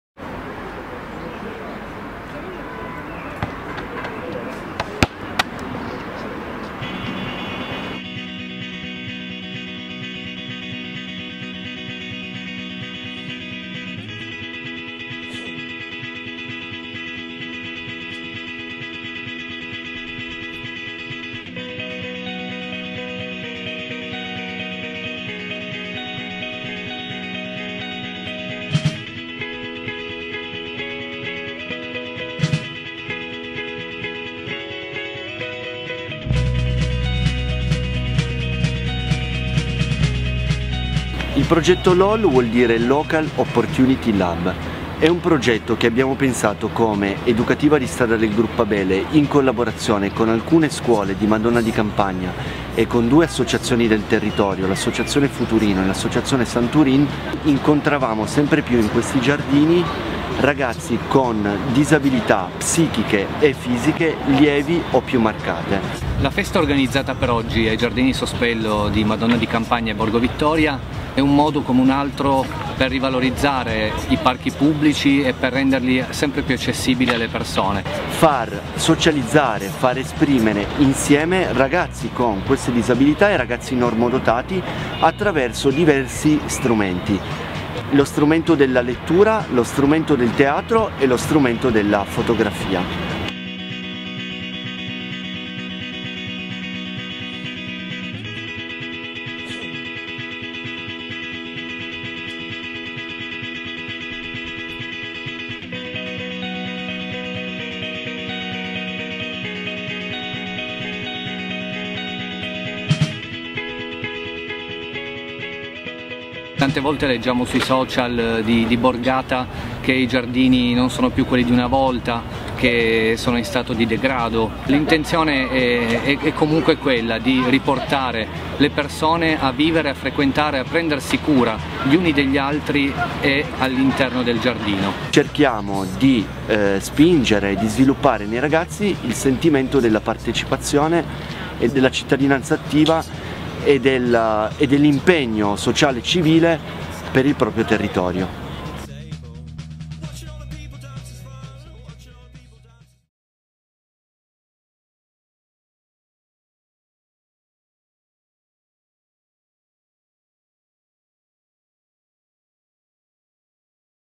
Tutti "diversi, uguali, unici, insieme". Borgo Vittoria in festa
Giovedì 16 maggio 2019, ai giardini don Gnocchi di via Sospello, a Torino, grande festa di partecipazione per ragazzi, promossa dall'Educativa di strada del Gruppo Abele insieme alle associazioni SanTourin e Futurino a conclusione del progetto LOL (Local Opportunity Lab). Nel video interviste a Nicola Pelusi (Gruppo Abele) e Diego Gallea (SanTourin).